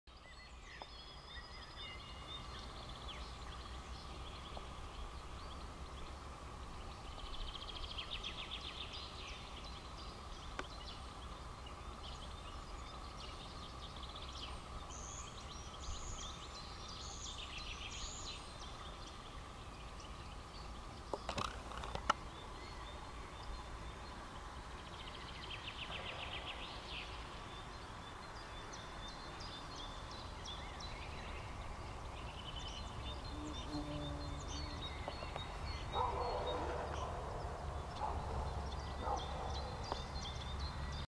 luňák červený, (Milvus milvus), v letu, 29. 3. 2014
Krouží u hnízda a přemýšlí, jestli mi ho má prozradit. Nakonec mi věří a prudká otáčka na konci videa je slétnutí k hnízdu.